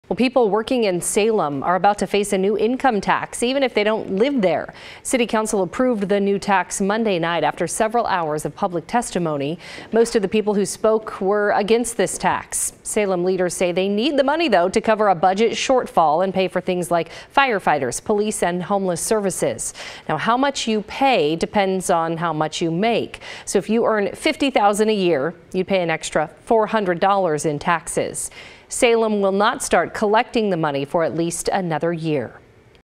Salem City Council approves new income tax
The Salem City Council approved the new tax Monday night after several hours of public testimony. The tax will also impact those who work but don't live in Salem.